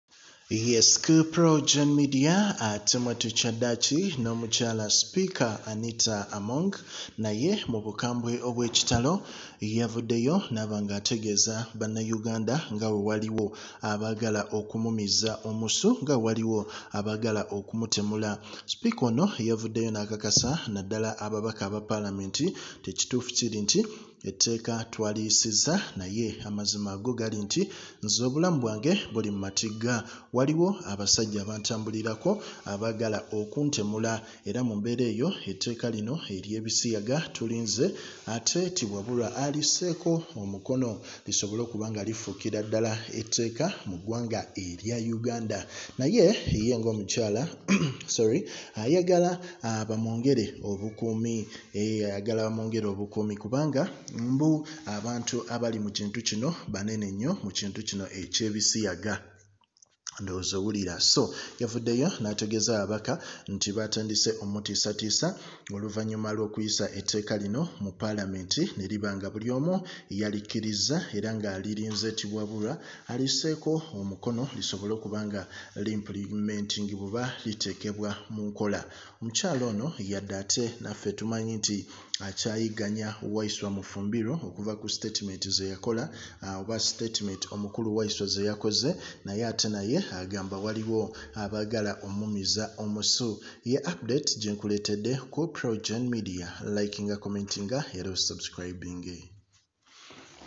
Okutemula Speaker Anita Among Bitiisiza Ababaka Eteeka lyebisiyaga btandise omutambulirako
Speaker Among Received Threats before Passing Anti-Homosexuality Bill.
The Speaker of Parliament Hon. Anita Among, has disclosed that she received a high volume of threats before the House approved the Anti-Homosexuality Bill, 2023.